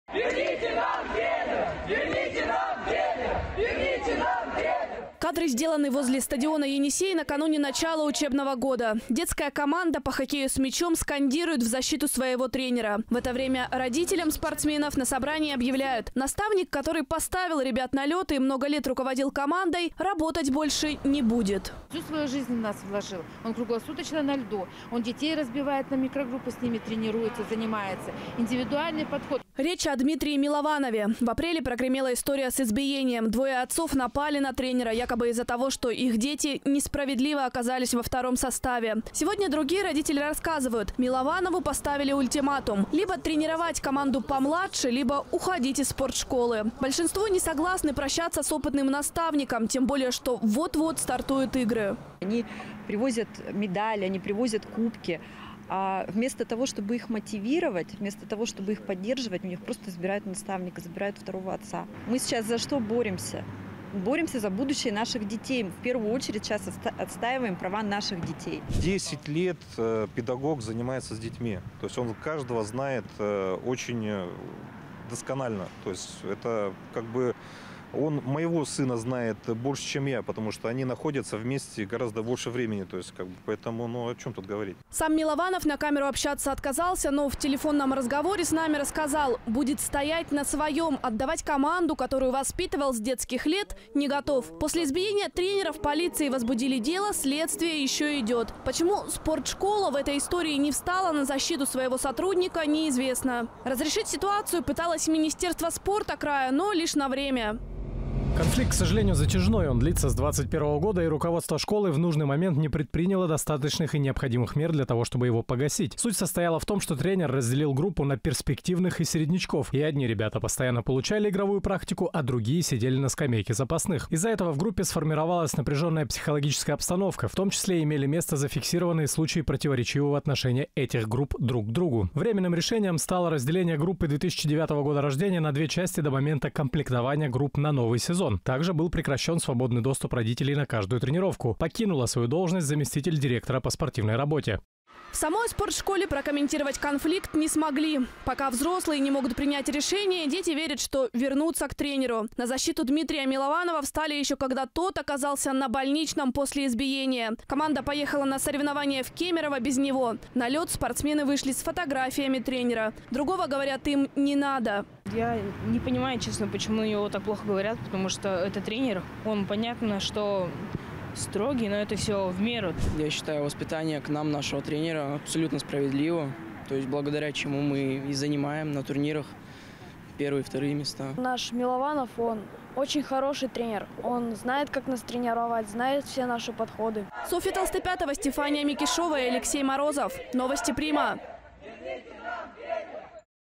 Избитого тренера по хоккею пытаются отстранить от работы
Тренера по хоккею с мячом, которого избили родители, теперь пытаются отстранить от работы с командой. Нападение произошло еще в апреле. Руководство спортшколы и министерство спорта вместо
разрешения конфликта разводят руками.

Кадры сделаны возле стадиона «Енисей» накануне начала учебного года. Детская команда по хоккею с мячом скандирует в защиту своего тренера. В это время родителям спортсменов на собрании объявляют — наставник, который поставил ребят на лёд и много лет руководил командой, работать больше не будет.

Речь о Дмитрии Милованове. В апреле прогремела история с избиением: двое отцов напали на тренера якобы из-за того, что их дети несправедливо оказались во втором составе. Сегодня другие родители рассказывают — Милованову поставили ультиматум: либо тренировать команду помладше, либо уходить из спортшколы. Большинство не согласны прощаться с опытным наставником, тем более что вот-вот стартуют игры.

Сам Милованов на камеру общаться отказался, но в телефонном разговоре с нами рассказал — будет стоять на своем — отдавать команду, которую воспитывал с детских лет, не готов. После избиения тренера в полиции возбудили дело, следствие еще идет. Почему спортшкола в этой истории не встала на защиту своего сотрудника — неизвестно. Разрешить ситуацию пыталось министерство спорта края, но лишь на время:

Конфликт, к сожалению, затяжной — он длится с 2021 года, и руководство школы в нужный момент не предприняло достаточных и необходимых мер для того, чтобы его погасить.
Суть состояла в том, что тренер разделил группу на перспективных и середнячков, и одни ребята постоянно получали игровую практику, а другие сидели на скамейке запасных. Из-за этого в группе сформировалась напряженная психологическая обстановка, в том числе имели место зафиксированные случаи противоречивого отношения этих групп друг к другу.
Временным решением стало разделение группы 2009 г.р. на две части до момента комплектования групп на новый сезон. Также был прекращен свободный доступ родителей на каждую тренировку. Покинула свою должность заместитель директора по спортивной работе.

В самой спортшколе прокомментировать конфликт не смогли. Пока взрослые не могут принять решение, дети верят, что вернутся к тренеру. На защиту Дмитрия Милованова встали еще когда тот оказался на больничном после избиения. Команда поехала на соревнования в Кемерово без него. На лёд спортсмены вышли с фотографиями тренера. Другого, говорят, им не надо.